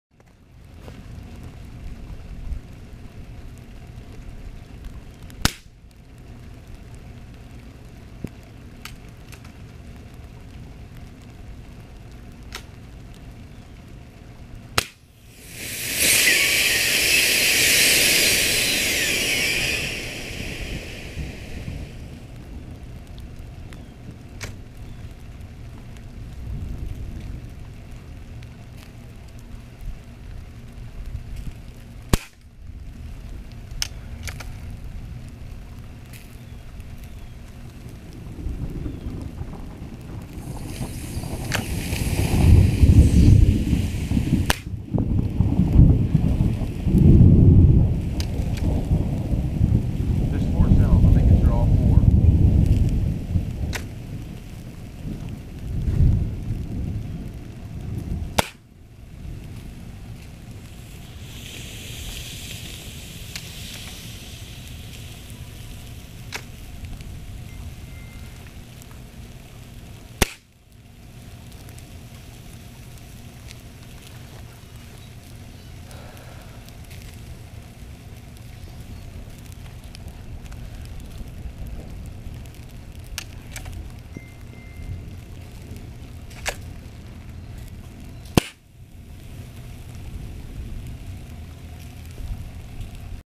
Lipo battery disposal.
Had a few puffed unbalanced packs and didn't want to just throw them in the trash. Used hollow point pellets and a break barrel pellet gun.